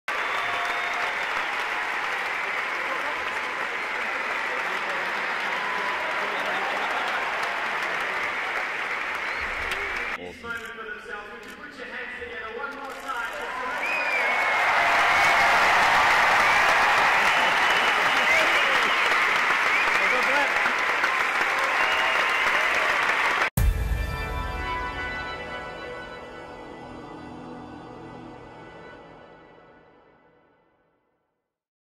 Serederer: A selfie for the ages | Mastercard Hopman Cup 2019
Team USA's Serena Williams and Team Switzerland's Roger Federer share a selfie after sharing the court at the Mastercard Hopman Cup.